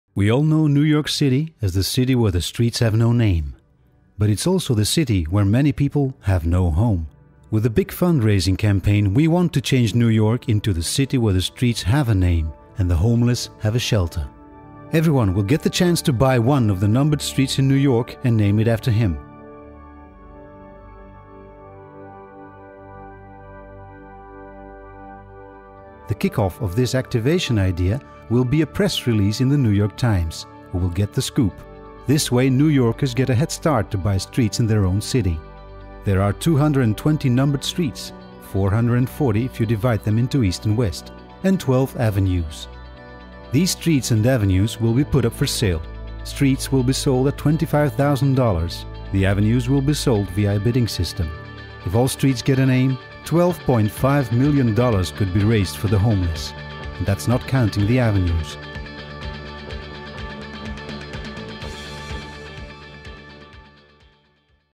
NewYork streets
Case film for Duval guillaume Brussels campaign New York Streets.
Creative Director: Peter  Ampe

direction and motion design by We Shout Horses